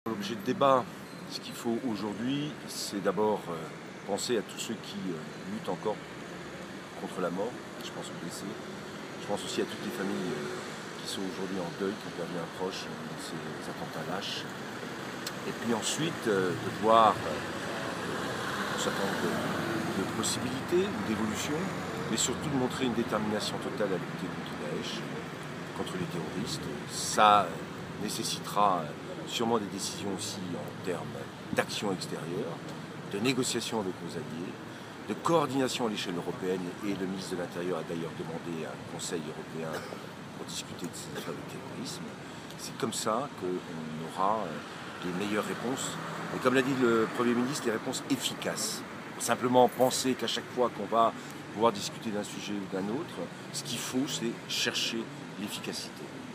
Réaction de Stéphane Le Foll aux attentats
Description : Présent lors d'un contrôle routier inopiné des forces de l'ordre dans le cadre de l'état d'urgence au Mans, Stéphane Le Foll réagit aux attentats